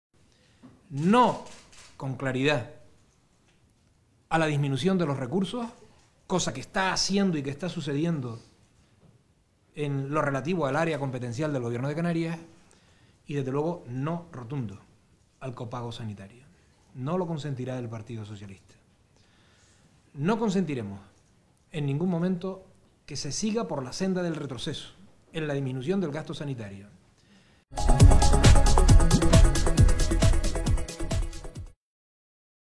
Los socialistas canarios rechazan la fórmula del copago
Los socialistas canarios han expresaro un rotundo "no" a la fórmula del copago en Canarias, y han subrayado que es un proyecto que nunca pondrán en funcionamiento si ganan las elecciones del 22 de mayo. Además han denunciado lo que consideran un continuo recorte en el presupuesto destinado a la sanidad pública canaria.
Habla: José Miguel Pérez, Candidato PSOE Presidencia